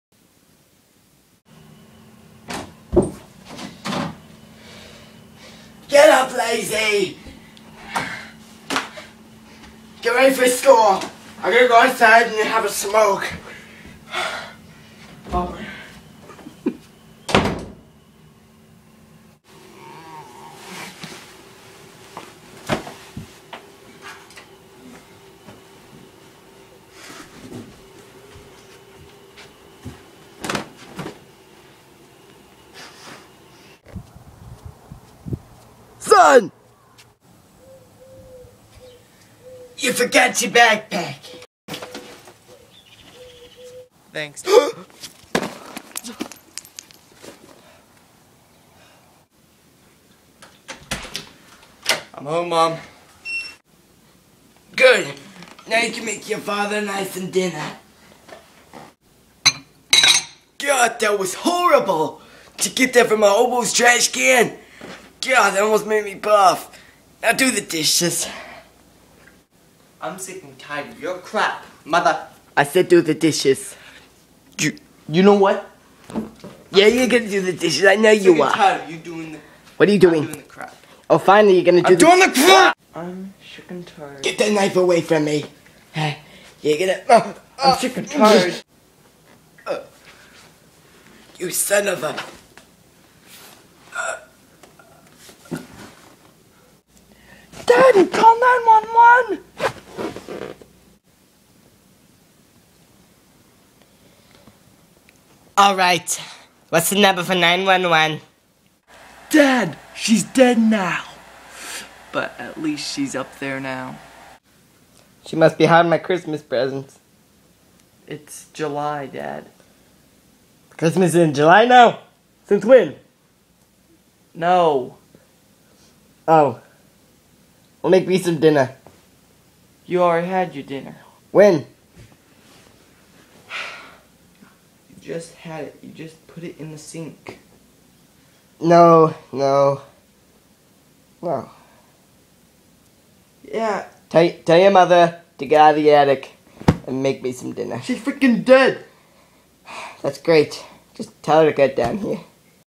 Horrible Mom Dies
When a horrible mom overworks her middle school son he loses his temper and gives his pregnant mom a heart attack.